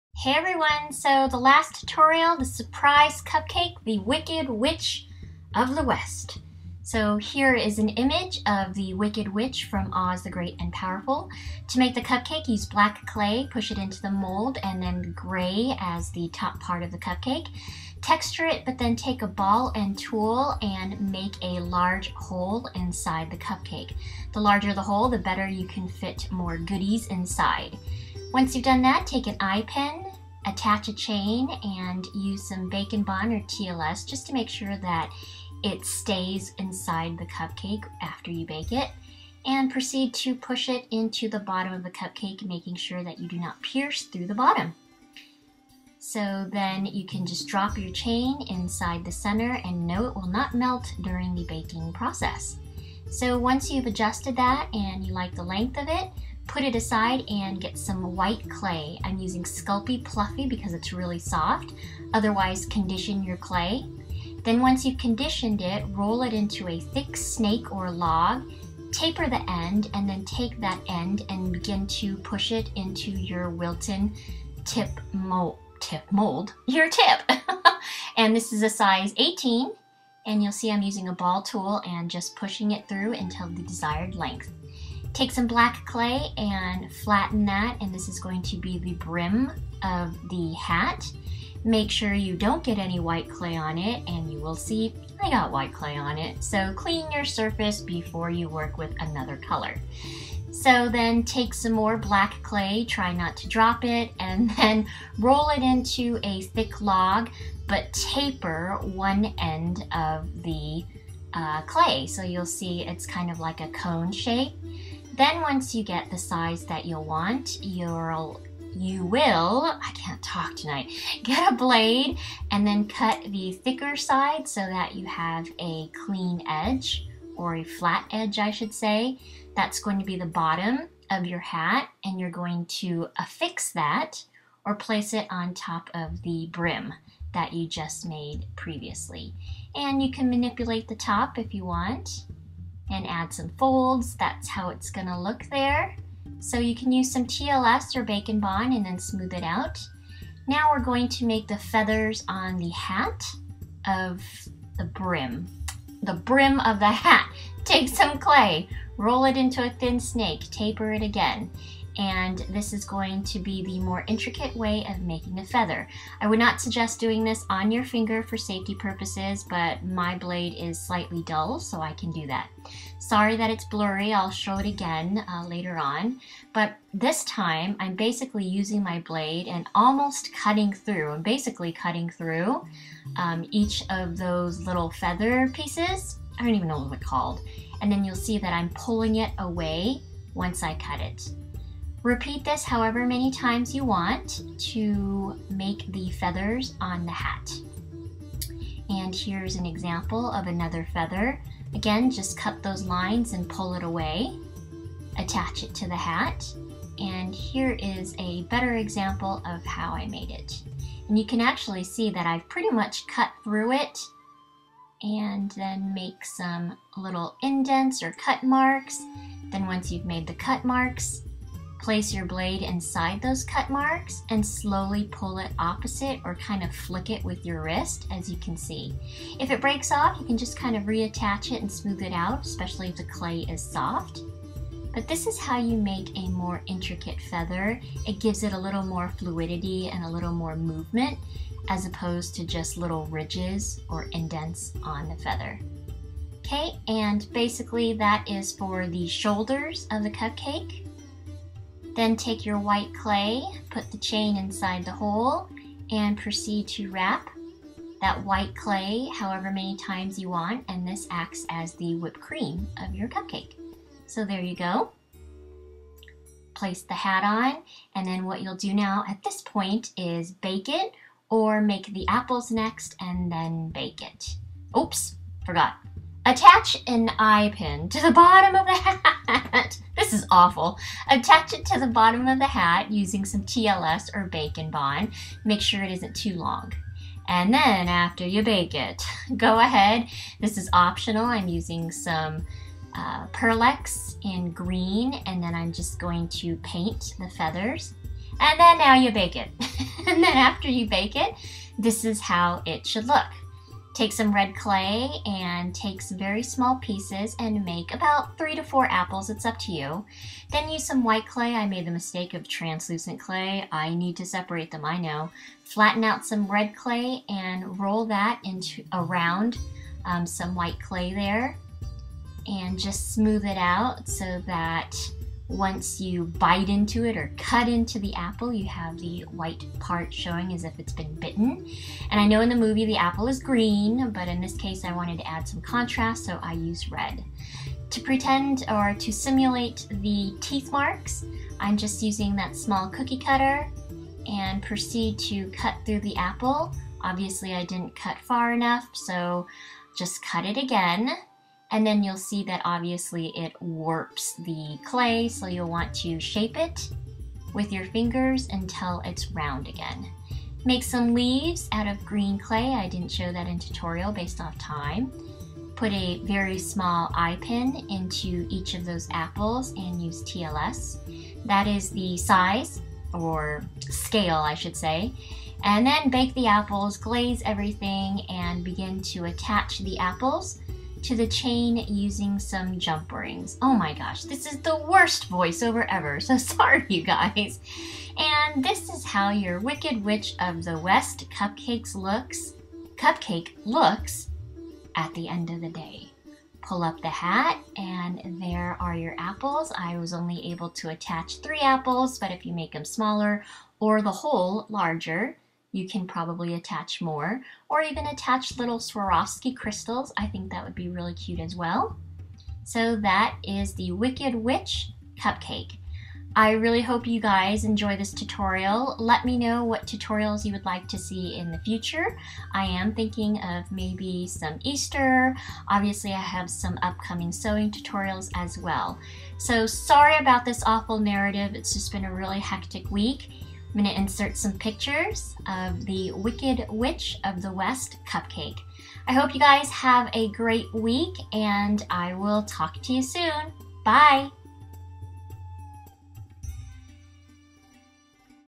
OZ The Great & Powerful: Surprise!! It's the Wicked Witch Cupcake Tutorial
Expand for Materials Used and Steps:

Materials:
-Any brand green, black, white, and red clay. I used Sculpey Pluffy clay in white as its soft and easy to condition / push through the icing tip.
-Toothbrush or sandpaper for cupcake texture.
-Cupcake base & top. Mine were purchased on Etsy from Miniature Sweets.
-6 eye pins (one long for the hat, one medium for the cupcake base, and 4 for the apples).
-Wilton #18 star tip.
-Roller / clear stamper to flatten clay.
-Any brand glaze. I used Americana glossy.
-Sculpey Bake and Bond or TLS to use as glue.
-Ball end tool (Sculpey) or anything that will assist in pushing the clay through the icing tip.
-5 jump rings (one to connect the chain to the hat and four for each apple).
-Blade to cut & add details to your feathers.
-Optional pearlex in green to add dimension to your feathers.
-Any glaze. I use Americana Satin.

Frequently Asked Questions:

Q: Why didn't you show the steps to make the feathers on the shoulder?
A: Because it's pretty much the same steps for the hat. Just make 3 feathers for each shoulder (6 total), overlap them and place them on each side of the cupcake.

Q: What is the significance of the apples?
A: I don't want to spoil it for those who have not seen the movie and if you asked, you probably haven't, but it plays a huge part of the Wicked Witch's transformation.

Q: Where did you purchase all of your tools and materials?
A: I purchase all of my tools at Michael's & Joann Fabrics with the exception of the cupcake mold.

Q: Where did you purchase your cupcake mold?
A: Etsy from a shop called Miniature Sweets.

Q: How long do you bake your cupcake for?
A: I bake mine for about 15 minutes in a toaster oven at 250 degrees Fahrenheit.

Q: What video editor do you use?
A: Apple iMovie. Still learning this darn thing. Tips?

Q: Are your trades open?
A: No, I don't trade anymore.

A: To record the video, I use a Samsung QIO and for photographs, a Nikon D90 with a Tamron Macro lens.

Q: Why didn't you upload this video yesterday?
A: I was trying to build tension and anticipation like a good movie...Did you buy that? Didn't think so. I've been going through some personal changes in my life right now, so yesterday was just one of those days... I'm ok though and so is John (my fiancée). We are OK!! :)